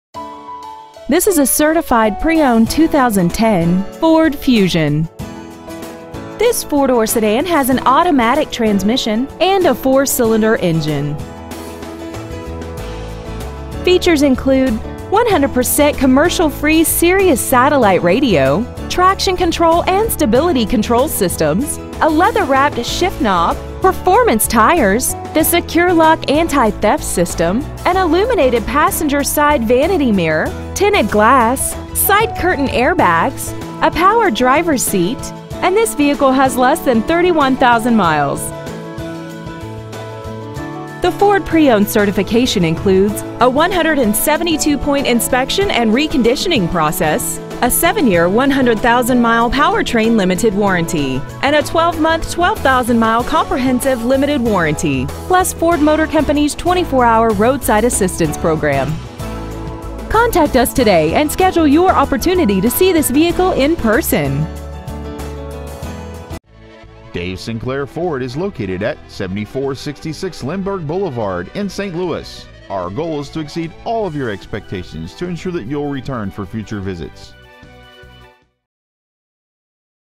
2010 Ford Fusion Certified St. Louis MO
Year : 2010
 Make : Ford
 Model : Fusion
 Engine : 2.5L
 Trans . : automatic
 Exterior : Red Candy Metallic Tinted - (Red)
 Miles : 30,948
 Interior : Charcoal Black Cloth
 Stock : P12129200

 7466 Dave Sinclair Memorial Highway

 Preowned 2010 Ford Fusion St. Louis MO